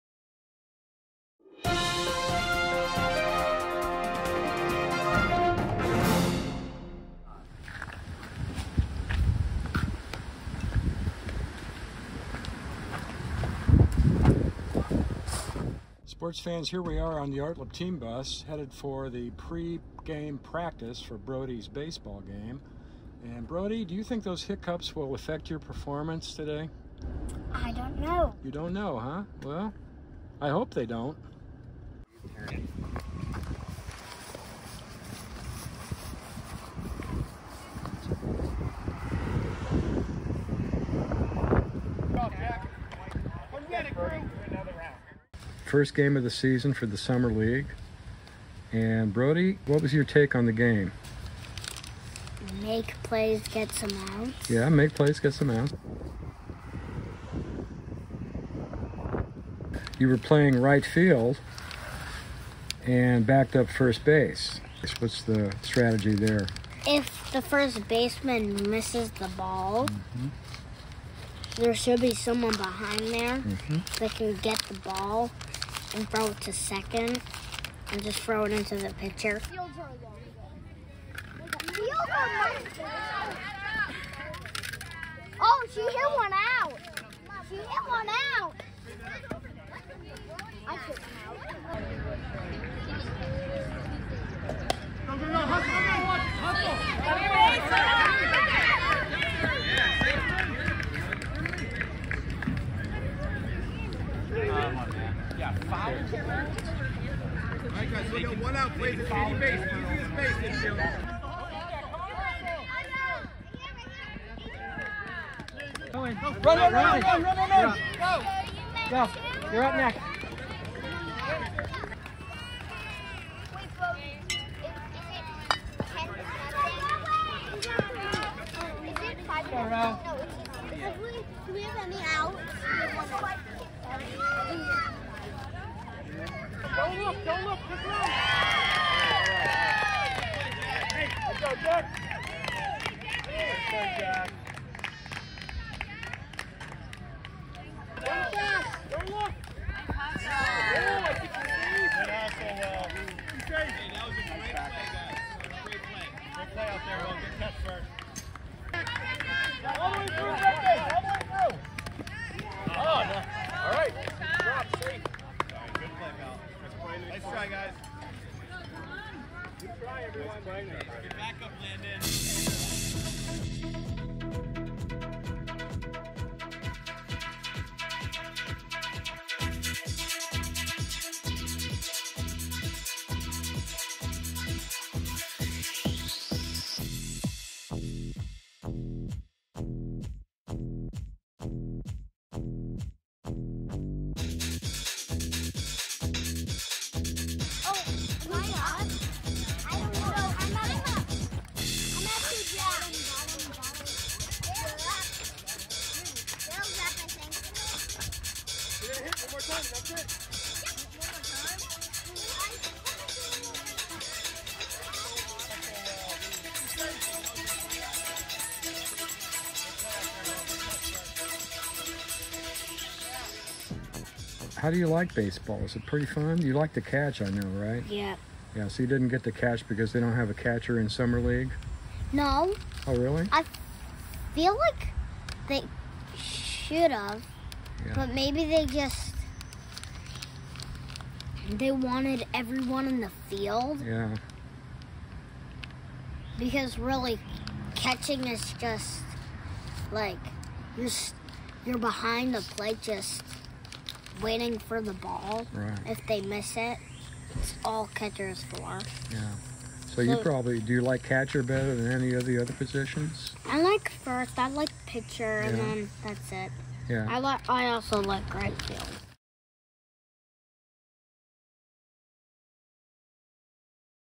Summer baseball 2023
After a stint in spring ball, Brody makes his summer league debut.